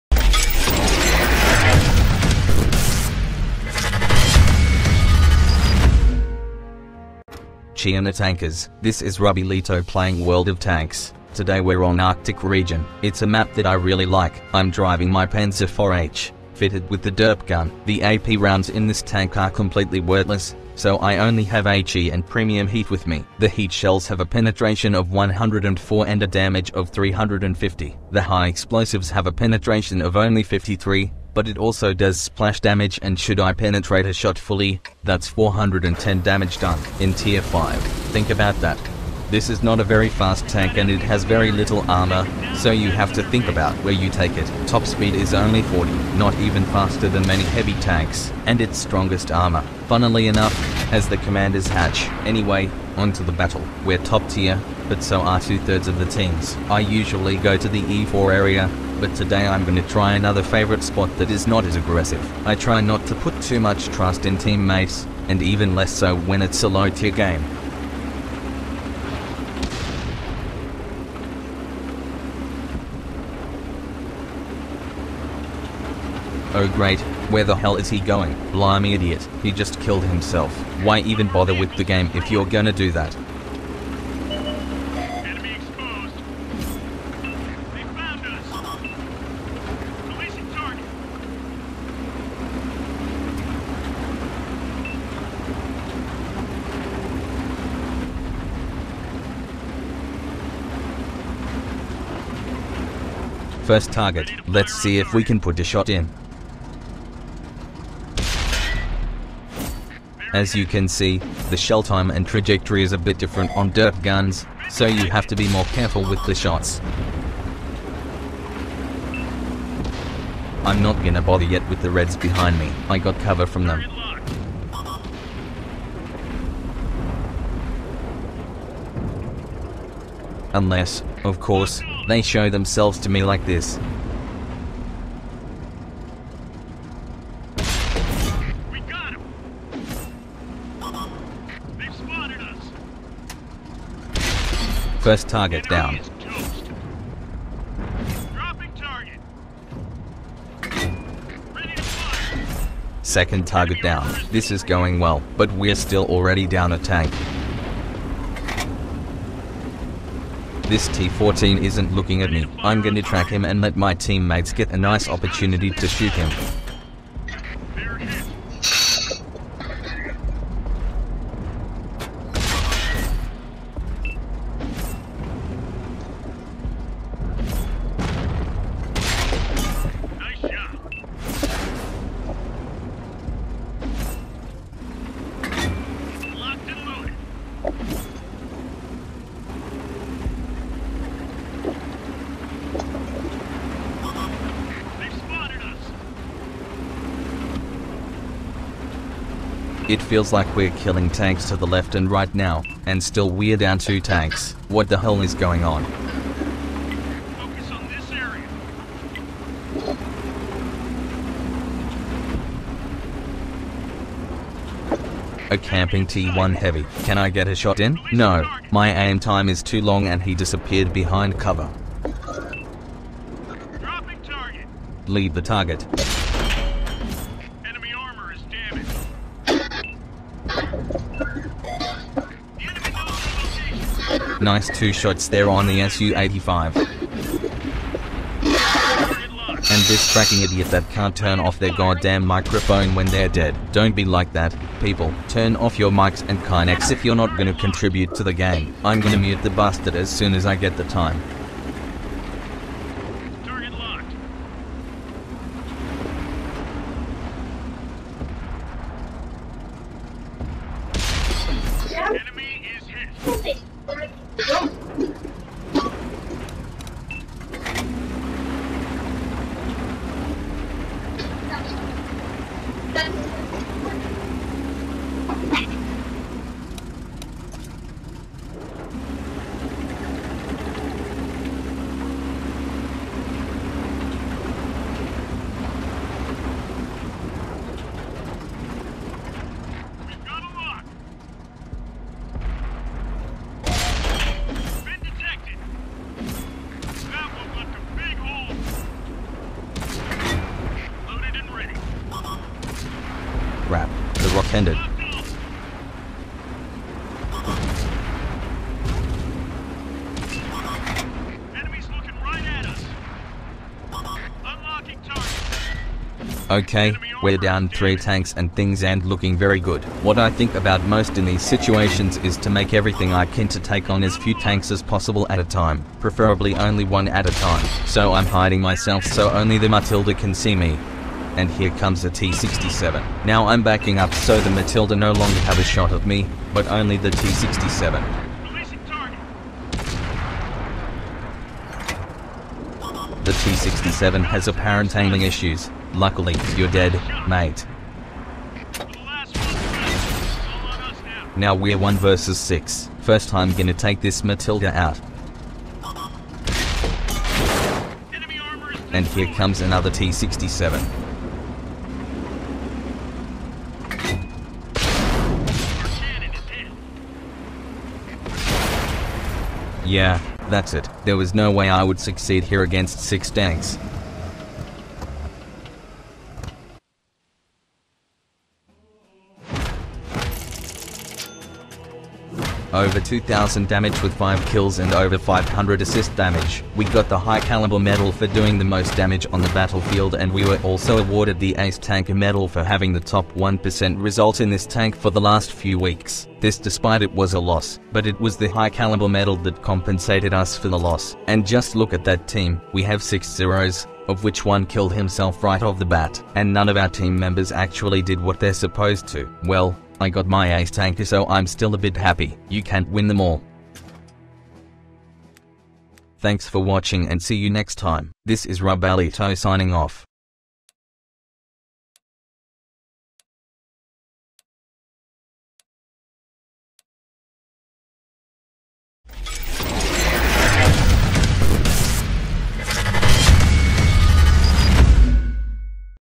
Pz IV H Derp gun - Ace tanker - Commentary
Pz. Kpfw. IV Ausf. H, that's the complete name of this tank.
I fitted it with the derp gun and here's how I got the Ace tanker in it.
I elited it, and then played a few more games because I wanted to get the 1st mark of Excellence in it before abandoning it for the next tank, so there will be no more videos with this tank in a foreseeable future.